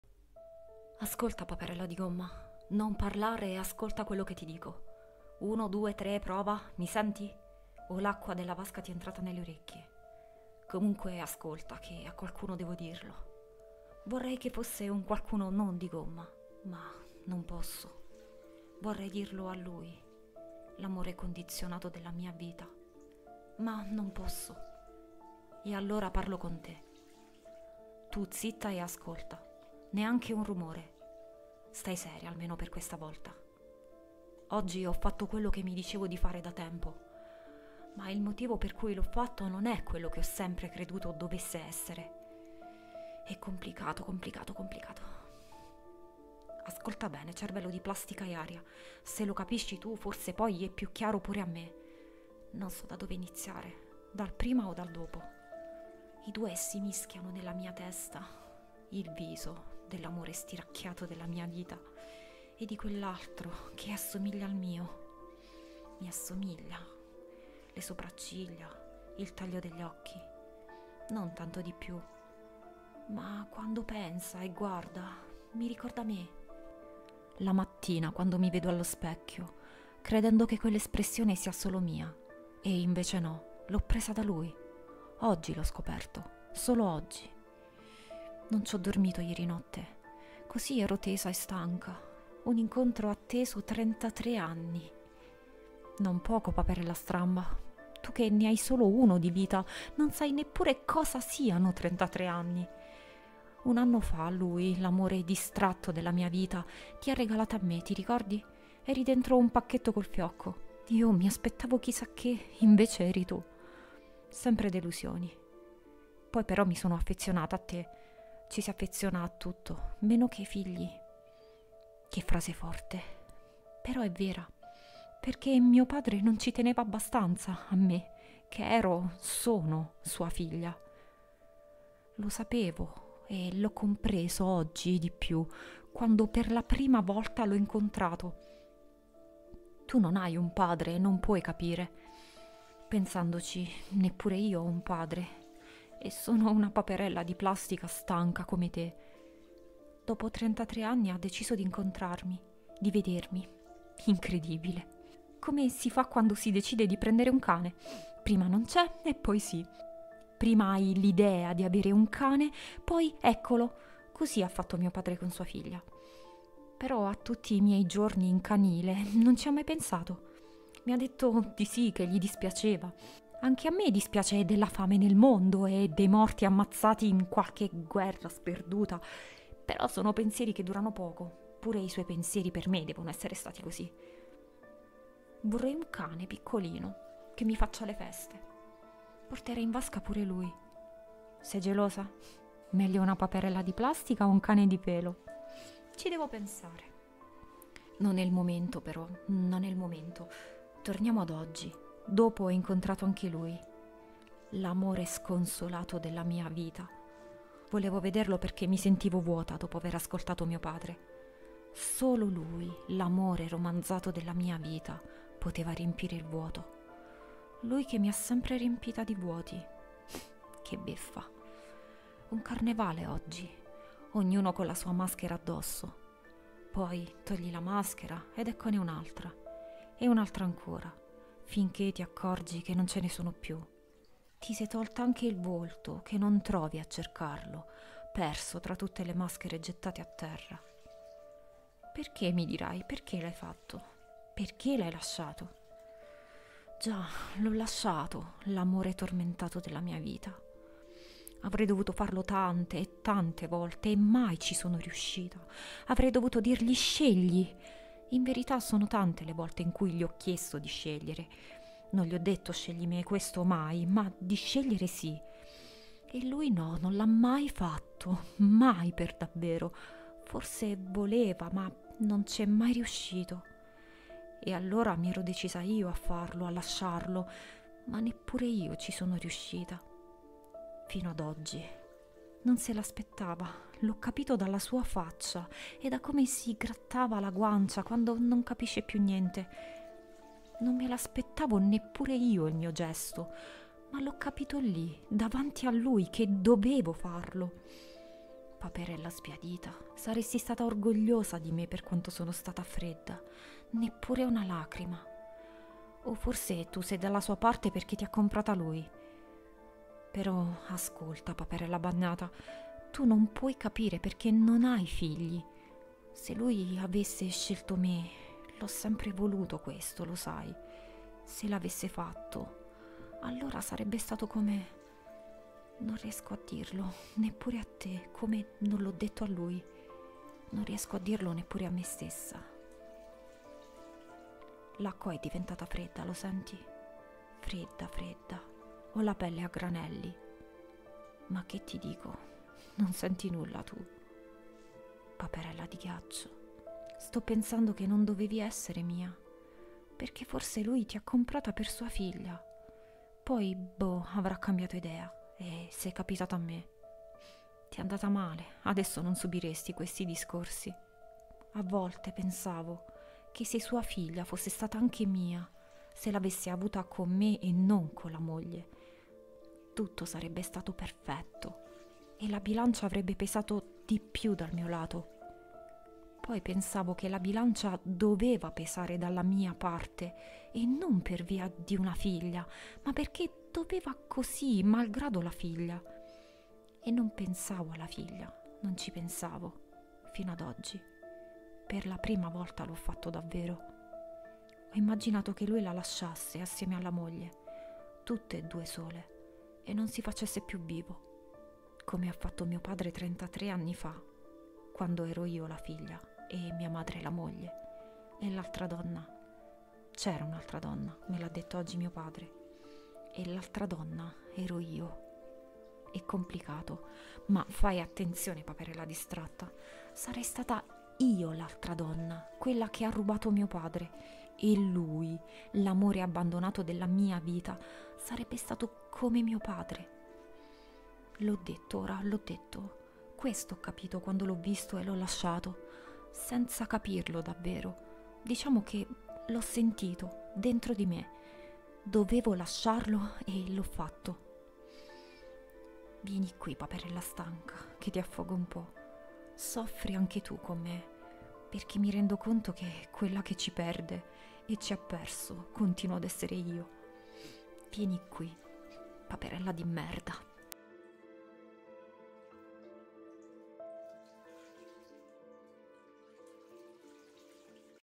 ASCOLTA PAPERELLA di Bruno Barcellan
Ci sono cose difficili da confessare, anche a se stessi... a volte ad ascoltare c'è solo una paperella di gomma... Ascolta paperella

Racconto: Bruno Barcellan
Lettura: Mariapia Zaccagnino